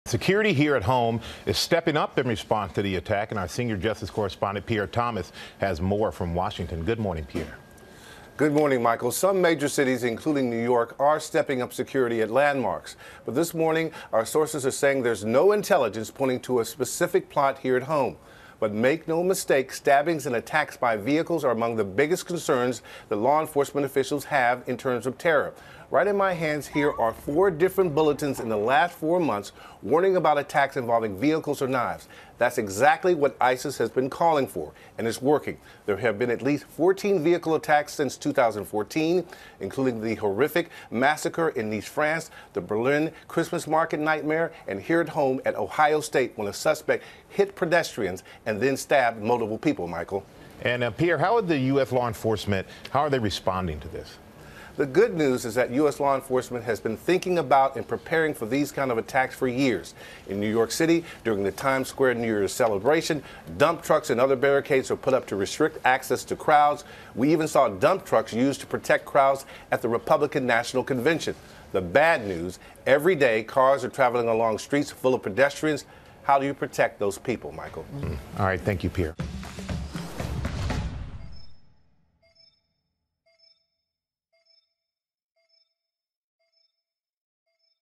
US cities increase security after deadly London attack
Authorities in major U.S. cities, including New York City, are stepping up security at landmarks after the terrorist incident at London's Westminster Bridge and Houses of Parliament.